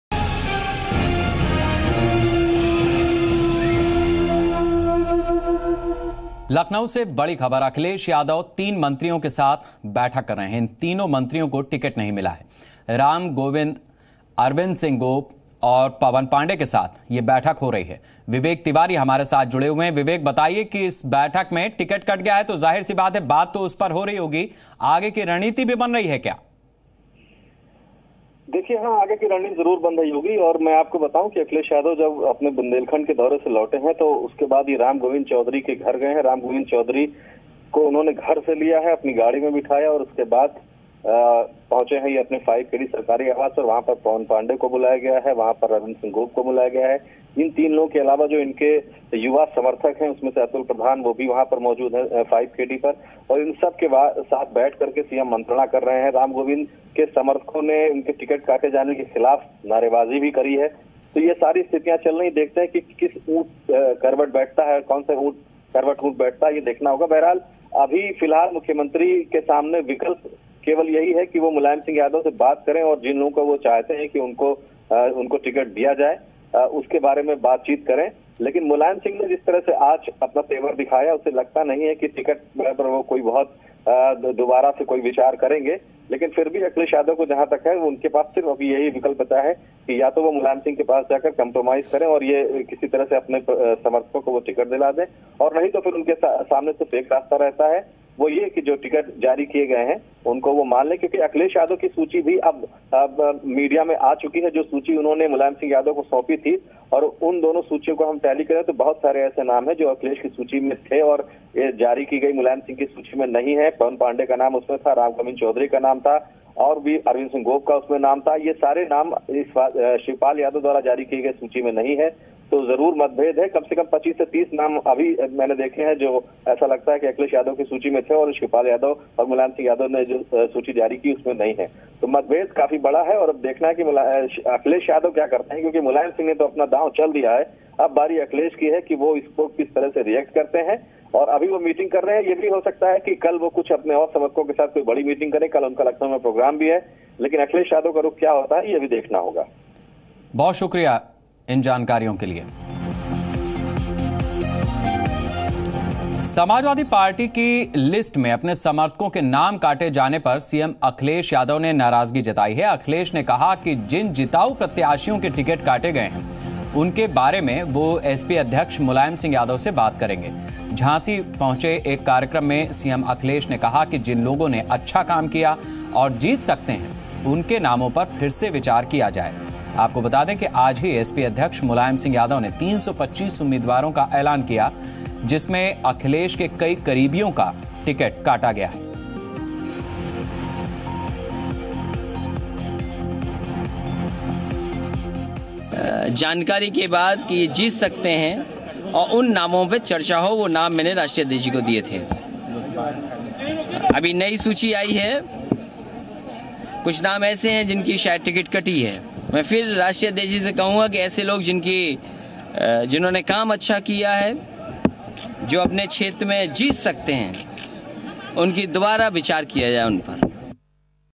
Akhilesh Yadav convened meeting with cabinet ministers
Akhilesh Yadav convened meeting with cabinet ministers. Watch this special segment and get to know more here.